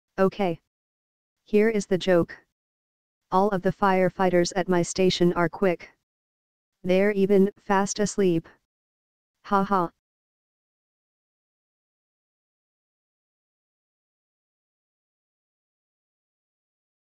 Ok, here is the joke. All of the firefighters at my station ar...
All of the firefighters at my station are quick. They're even "fast" asleep!